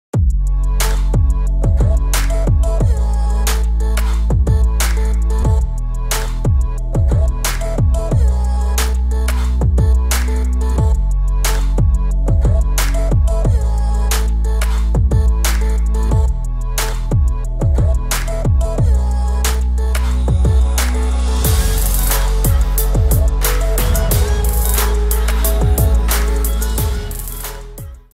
Set x3 BBQ ¡El complemento ideal para tu parrilla!
El set x3 BBQ son los encargados de preparar los mejores asados en tu hogar al lado de Universal 😎 ¿qué esperas? y sé el primero en tenerlos.